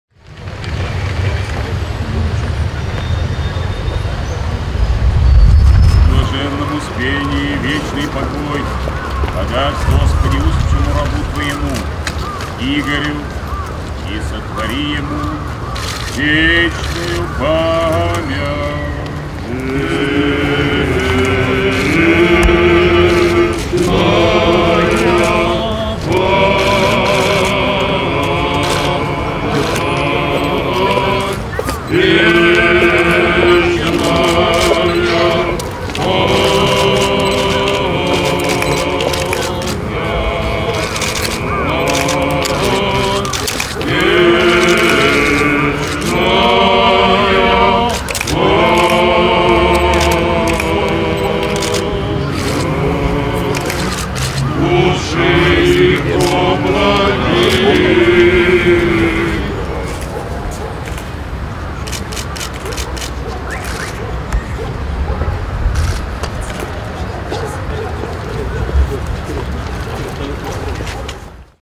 Патриарх помянул погибшего губернатора Сахалина
Южно-Сахалинск, 20 сентября 2010 года. По окончании всенощного бдения в Воскресенском кафедральном соборе Святейший Патриарх Московский и всея Руси Кирилл посетил мемориал-памятник с поклонным крестом, воздвигнутый на территории собора в память о трагедии 20 августа 2003 года — гибели в авиакатастрофе губернатора Сахалинской области И.П. Фархутдинова и четырнадцати других региональных руководителей.